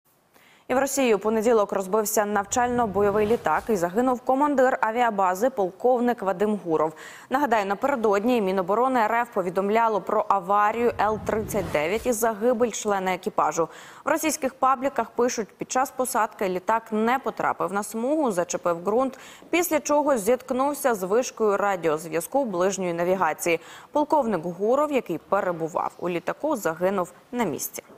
У Краснодарському краї РФ впав навчальний літак
У Краснодарському краї Росії зазнав авіакатастрофи навчальний літак.